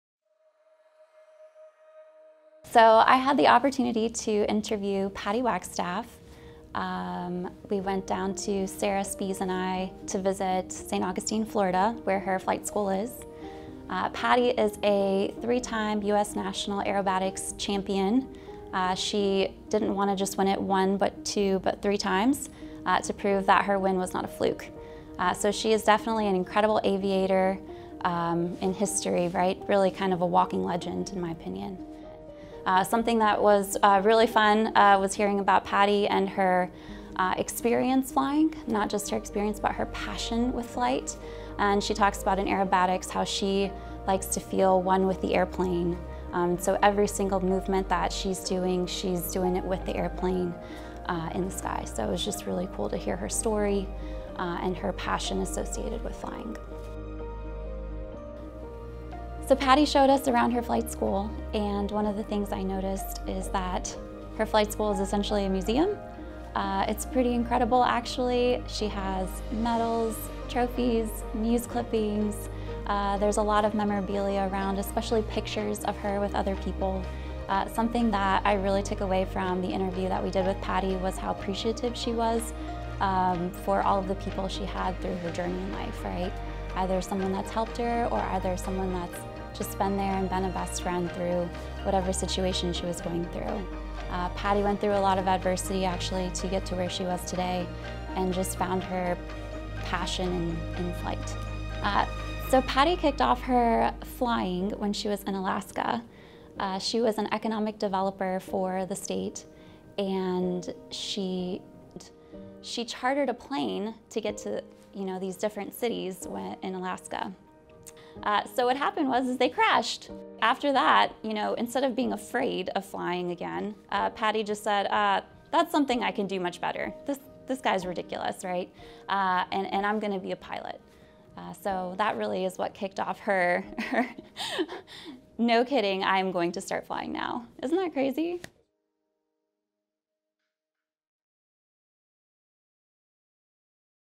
GOE 2023:Patty Wagstaff
Patty Wagstaff was selected as an Eagle because she is a multi-time U.S. Aerobatic medal winner, winning gold, silver, and bronze medals in international competitions for several years. In 1991, she won her first of three US National Aerobatic Championships, the first woman to win that competition. She was the International Aerobatic Club champion in 1993. The following year, her Goodrich-sponsored Extra 260 airplane was put on display next to Amelia Earhart’s Lockheed Vega at the Smithsonian Institution’s National Air and Space Museum. From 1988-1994, she won the Betty Skelton First Lady of Aerobatics award six times in a row. In 1996, Wagstaff was the top-scoring US pilot at the World Aerobatics Championship. In 2002, she won the Katherine and Marjorie Stinson Award. In 2004, she was elected into the National Aviation Hall of Fame.